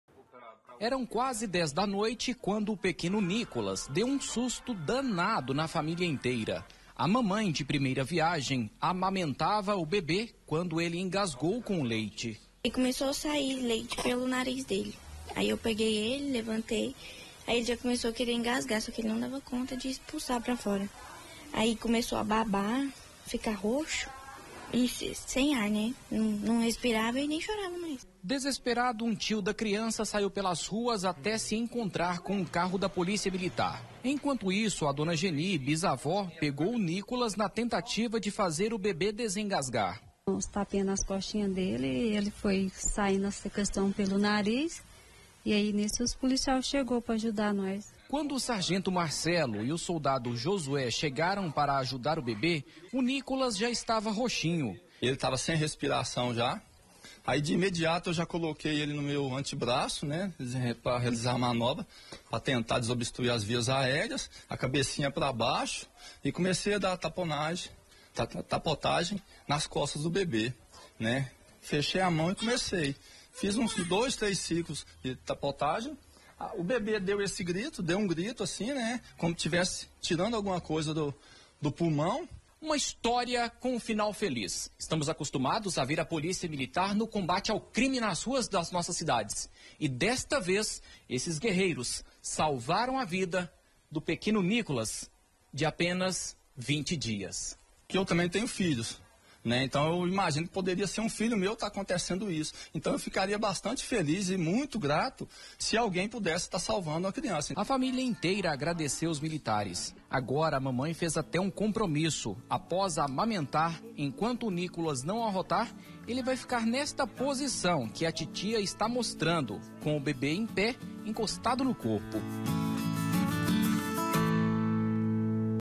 FINAL FELIZ: POLICIAIS SALVAM CRIANÇA ENGASGADA
A experiência de dois policiais militares ajudou a salvar um bebê, de apenas 20 dias, em Anápolis. A criança estava engasgada e a família desesperada. Por sorte, os militares estavam por perto e conseguiram reanimar o bebê.